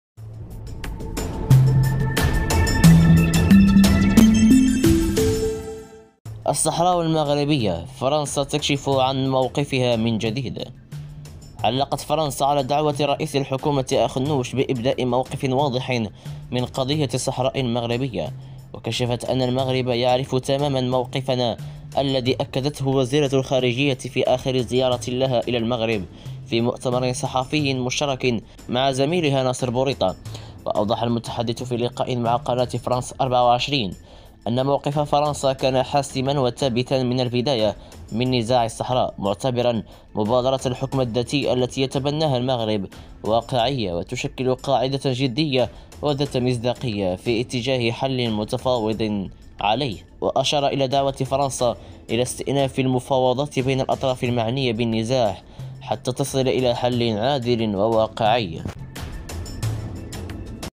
الصحراء المغربية.. فرنسا تكشف من جديد عن موقفها بخصوص الصحراء
الصحراء المغربية.. فرنسا تكشف عن
موقفها من جديد
فرنسا والصحراء المغربية
علقت فرنسا على دعوة رئيس الحكومة عزيز
أخنوش، بإبداء موقف واضح من قضية الصحراء
المغربية.

وكشفت أن “المغرب يعرف تماما موقفنا، الذي
أكدته وزيرة الخارجية في آخر زيارة لها إلى
المغرب في مؤتمر صحافي مشترك مع زميلها ناصر
بوريطة".